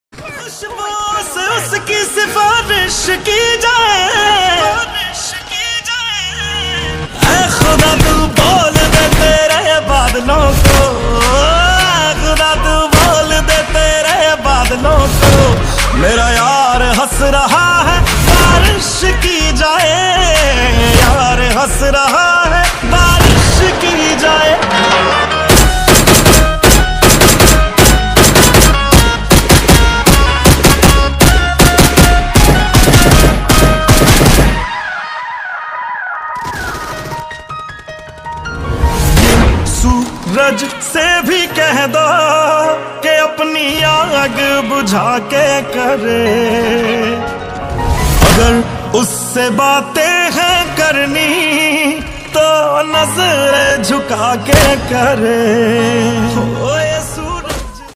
Omg ! No Death in TDM with Only M24😱| Become tdm Beast | FT• बारिस की जाए
🎯BECOME TDM BEAST
📱ASUS ROG 3 (ANDROID) 90fps
🎧 COSMIC BYTE
🗼MOBILE DATA

Tags :- @JONATHANGAMINGYT @MortaLyt @DynamoGaming @CarryMinati @CarryisLive @AntaryamiGaming @ZGODGAMING @NEYOOGAMINGYT @goblinbgmi